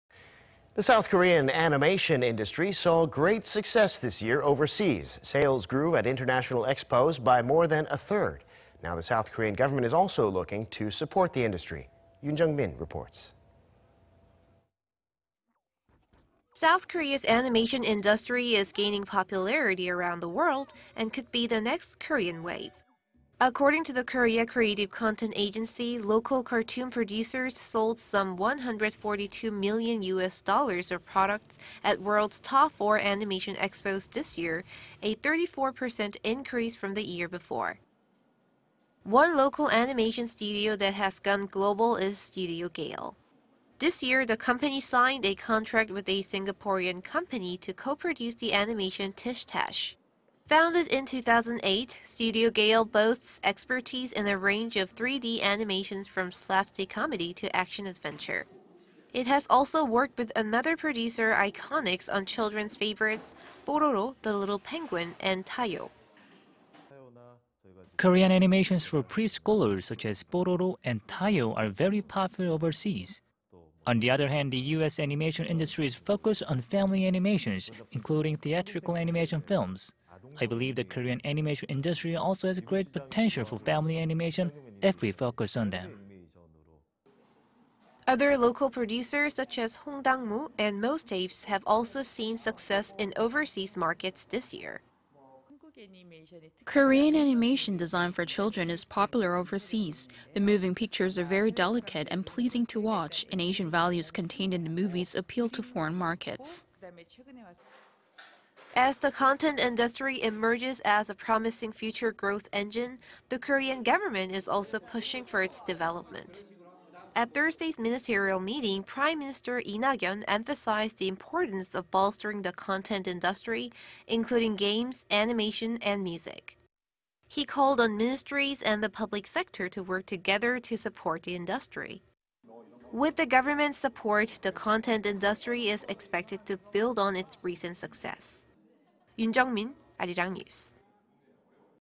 Animation shows potentials for future content industry, adds flavor to Korean Wave
애니메이션, 즐거운 상상력으로 새로운 한류를 이끌다

The South Korean animation industry saw great success this year overseas.
Sales grew at international expos by more than a third.
The Korean government is also looking to support the industry.
Yoon Jung-min reports.
South Korea's animation industry is gaining popularity around the world and could be the next 'Korean Wave'
According to the Korea Creative Content Agency, local cartoon producers sold some 142 million U.S. dollars of products at world's top four animation expos this year, a 34 percent increase from the year before.
One local animation studio that has gone global is Studio Gale. This year, the company signed a contract with a Singaporean company to co-produce the animation "Tish Tash."
Founded in 2008, Studio Gale boasts expertise in a range of 3D animations from slapstick comedy to action adventure.
It has also worked with another producer Iconix on children's favorites "Pororo, the Little Penguin" and "Tayo."
"Korean animations for pre-schoolers, such as  Pororo and  Tayo, are very popular overseas. On the other hand, the U.S. animation industry is focused on family animations including theatrical animation films. I believe the Korean animation industry also has great potential for family animations if we focus on them."
Other local producers, such as  Hongdangmoo and   Mostapes, have also seen success in overseas markets this year.
"Korean animation designed for children is popular overseas. The moving pictures are very delicate and pleasing to watch,... and Asian values contained in the movies appeal to foreign markets."
As the content industry emerges as a promising future growth engine, the Korean government is also pushing for its development.
At Thursday’s ministerial meeting, Prime Minister  Lee Nak-yon emphasized the importance of bolstering the content industry,... including games, animation and music.
He called on ministries and the public sector to work together to support the industry.
With the government's support, the content industry is expected to build on its recent success.